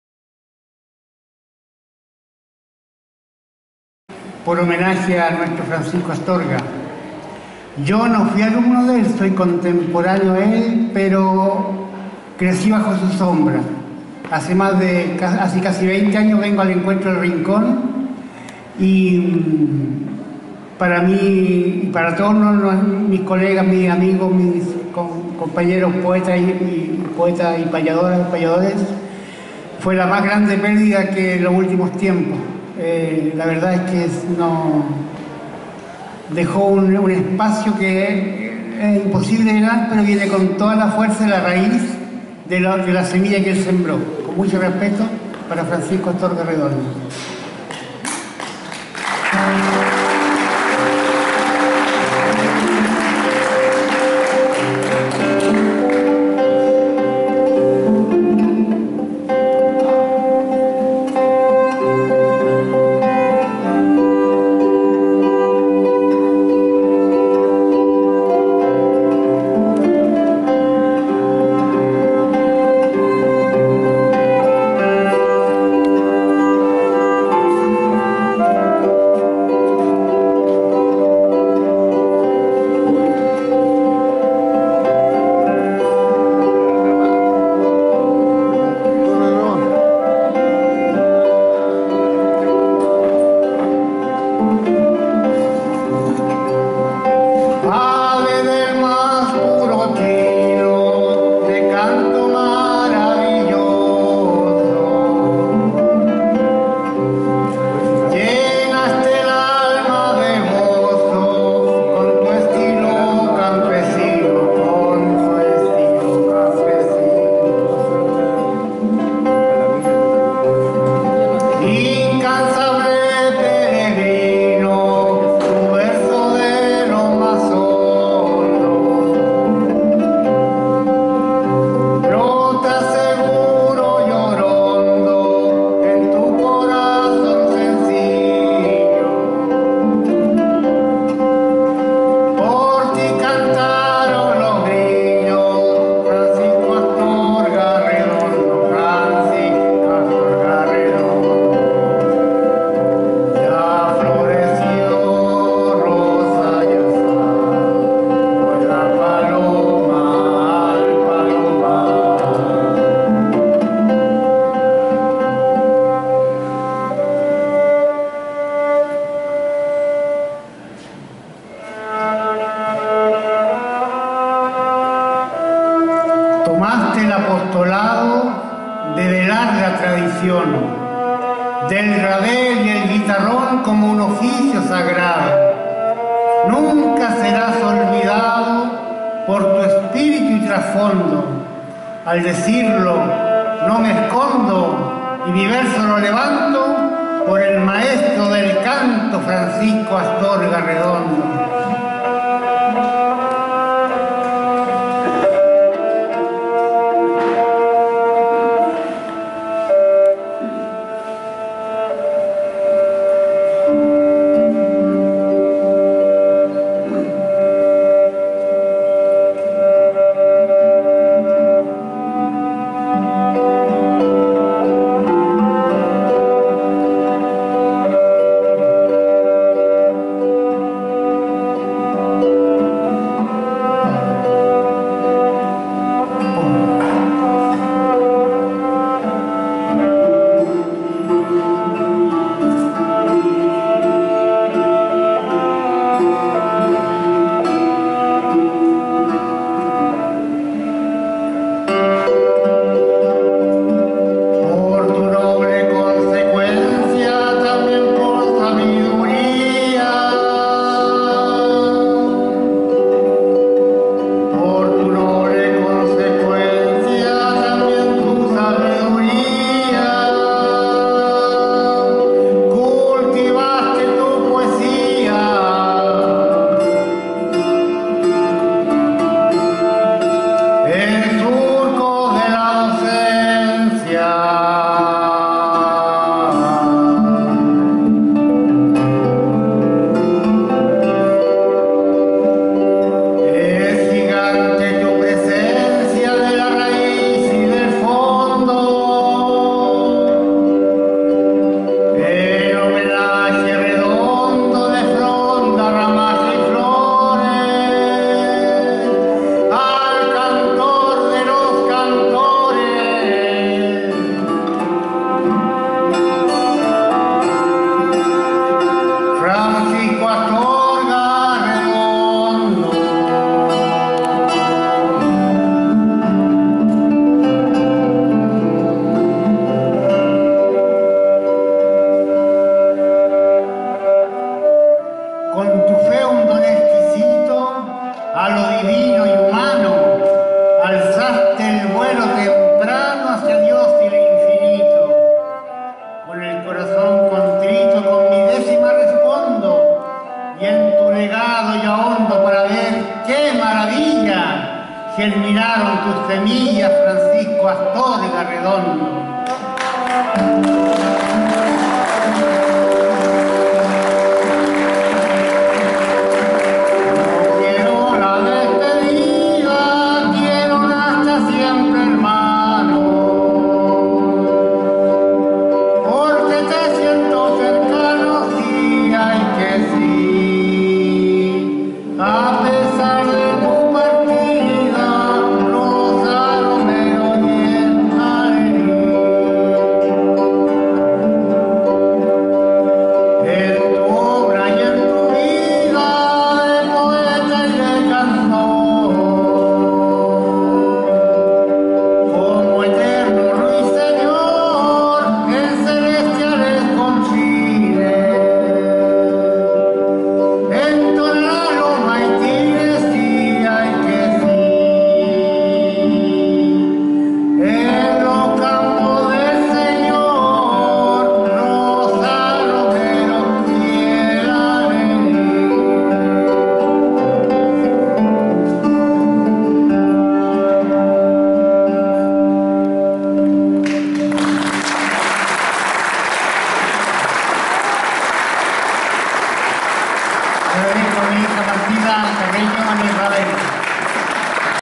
LUIS CARREÑO Y MARTINA CARREÑO / HOMENAJE A FRANCISCO ASTORGA , ENCUENTRO EL RINCON 2023
EMOTIVOS VERSOS E INTERPRETACION DE LUIS CARREÑO EN GUITARRON Y VOZ Y SU HIJA MARTINA CARREÑO EN EL RABEL , DEDICADOS A NUESTRO AMIGO FRANCISCO ASTORGA ARREDONDO , EN EL ECNUENTRO DE PAYADORES DEL RINCON , ENCUENTRO CREADO Y GESTIONADO  POR PANCHO Y SU FAMILIA DESDE SU NACIMIENTO , SIGUIENDO ESTA TRADICION AHORA SU FAMILIA , SU SEÑORA MIRYAM ARANCIBIA E HIJOS .